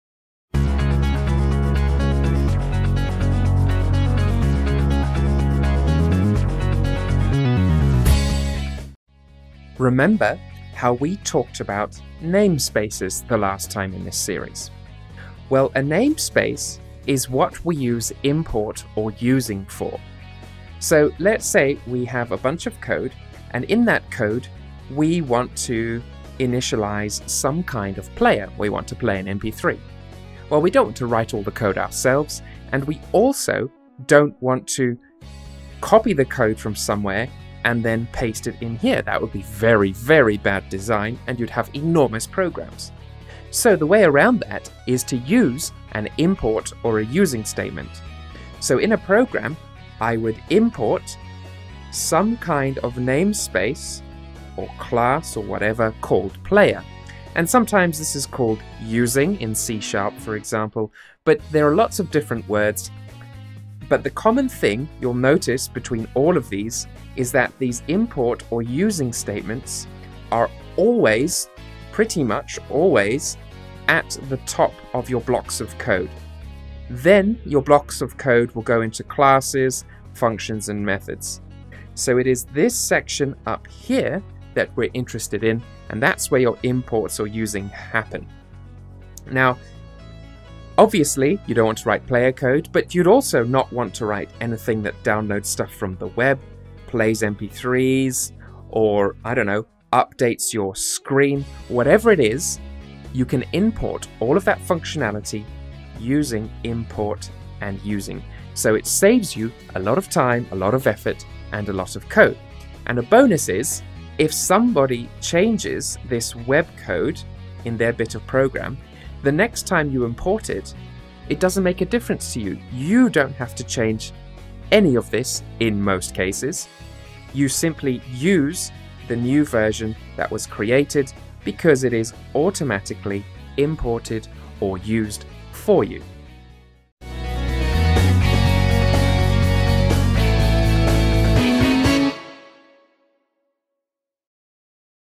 What does Import or Using mean in Programming? Day 7 of 365 Days of Programming
Programming goodness and explanations brought to you EVERY SINGLE DAY!